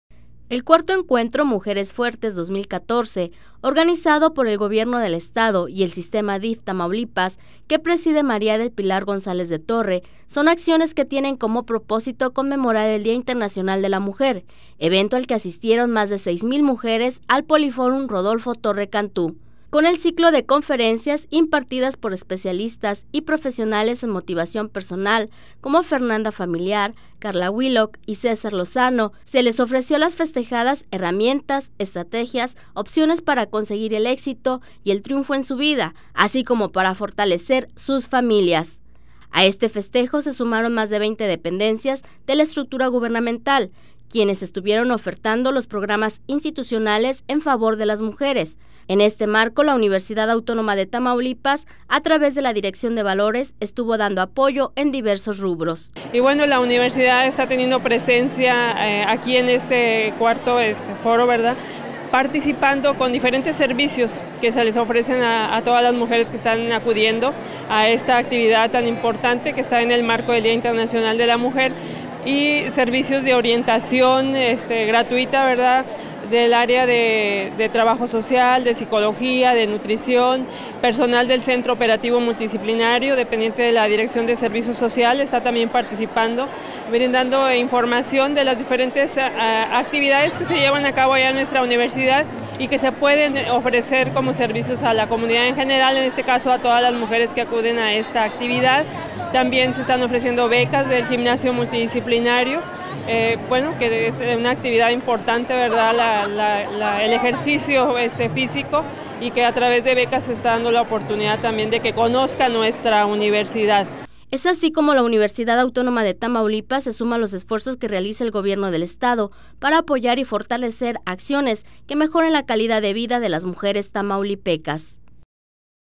Foro Mujeres Fuertes un escaparate para las mujeres tamaulipecas.
Con la presencia de especialistas en la motivación personal que disertaron diversos temas referentes a la participación de la mujer en nuestra sociedad tamaulipeca, el Foro Mujeres Fuertes se vio plagado de una alta afluencia femenina.